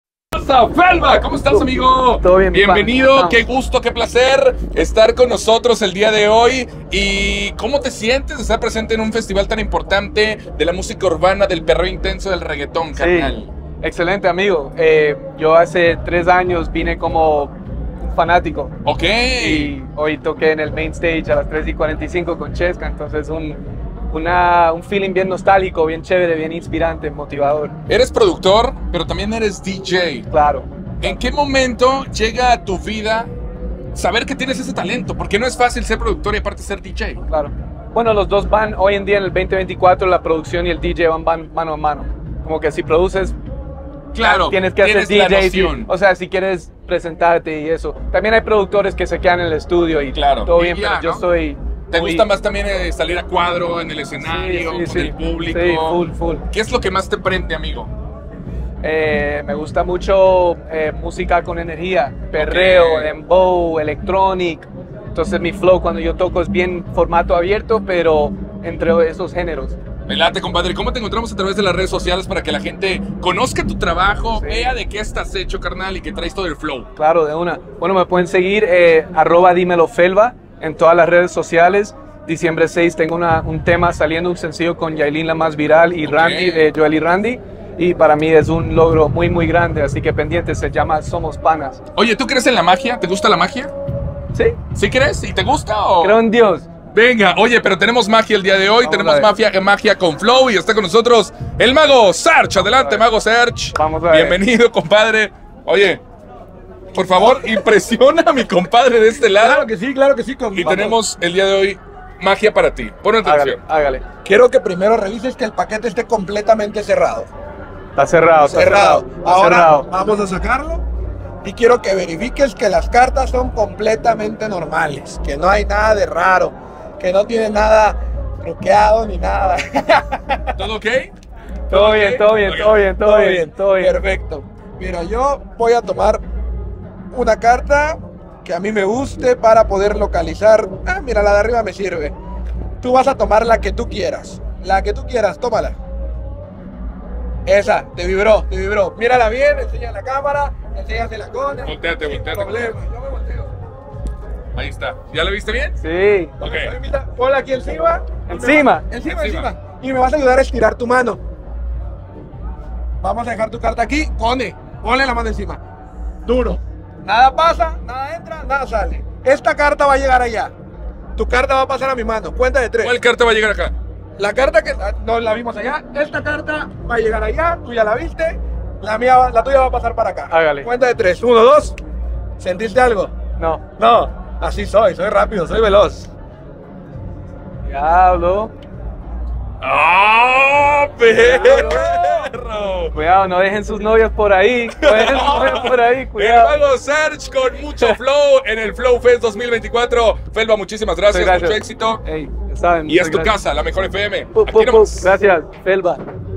FELVA con el mejor FLOW en el FLOW FEST LA MEJOR 🔥
Felva tiene todo el flow en el cocacolaflowfest y nos acompañó para contarnos su experiencia en el FlowFestLaMejor 😎